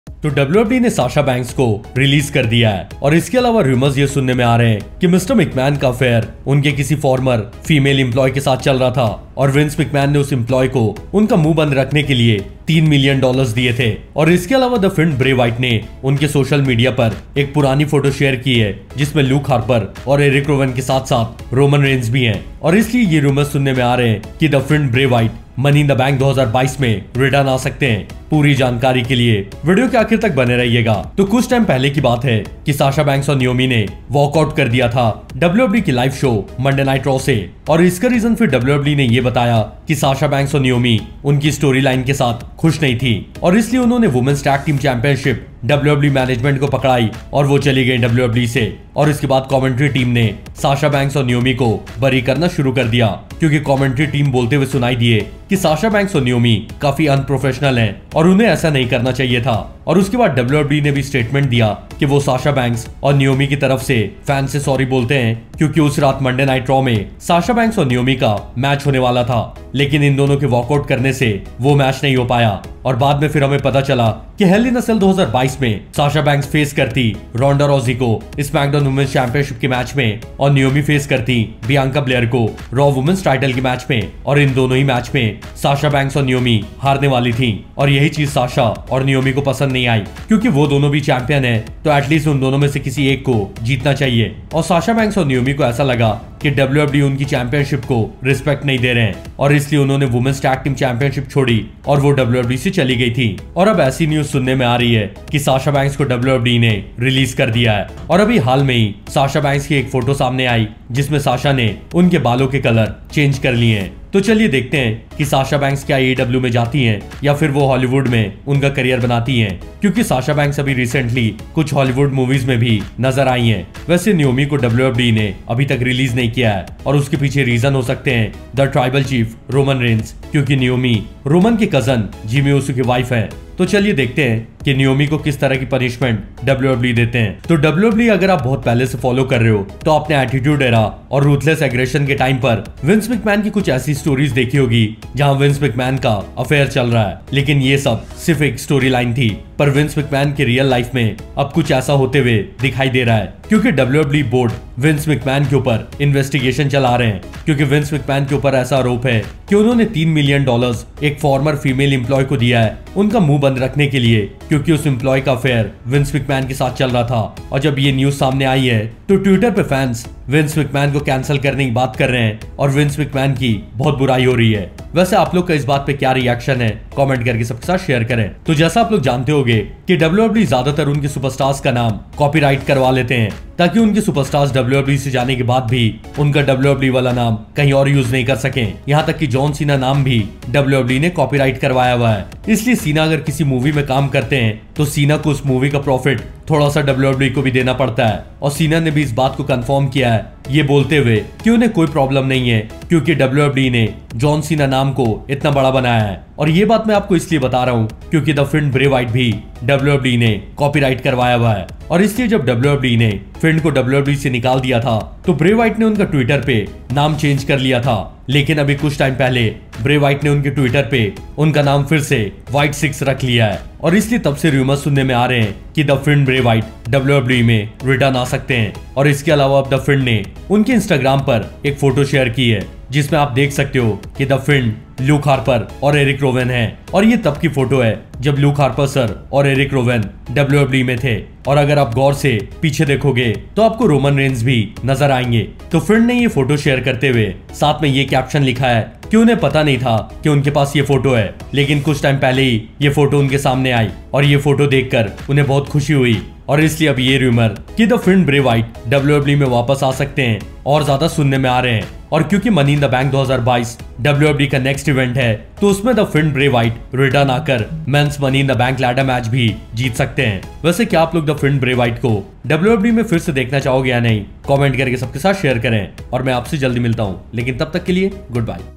The Fiend Bray Wyatt Shared Photo With Roman Reigns 2022
Watch the video to know about The Fiend & Roman Reigns.